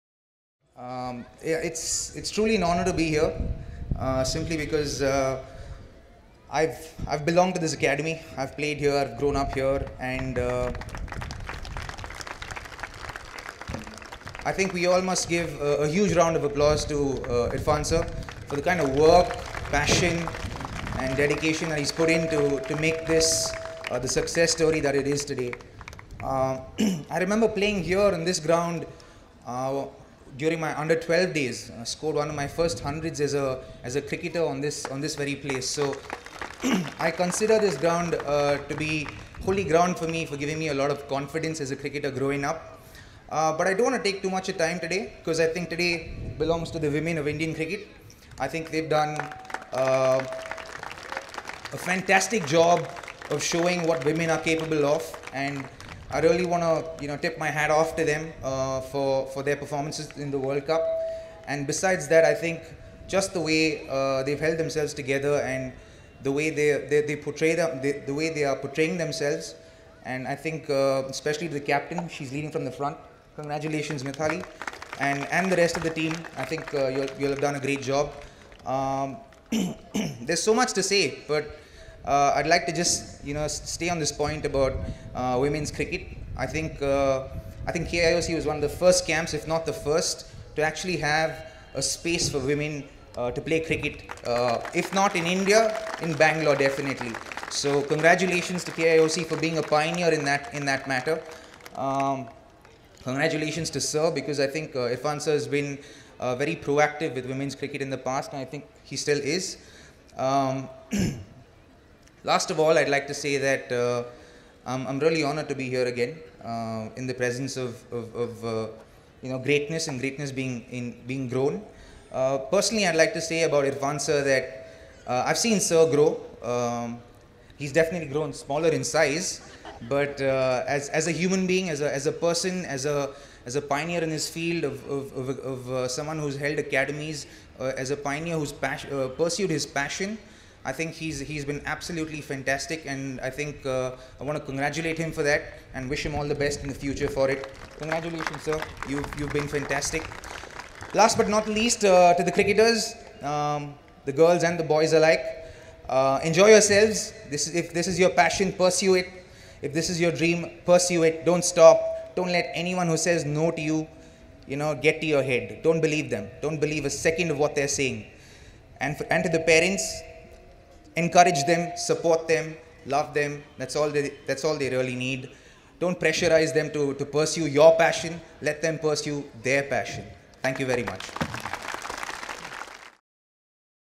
Believe in Yourself & Work Hard- Robin Uthappa to Young Cricketer
Uthappa addressing young cricketers and their parents at the inauguration of new KIOC campus.

Karnataka Institute of Cricket (KIOC) gets new campus for trainees. The new campus inaugurated in the presence of Indian women’s team skipper Mithali Raj, Veda Krishnamurthy, Robin Uthappa, C.M. Gautam, Nooshin Al Khadeer, Karuna Jain, V.R. Vanitha and many other cricketers.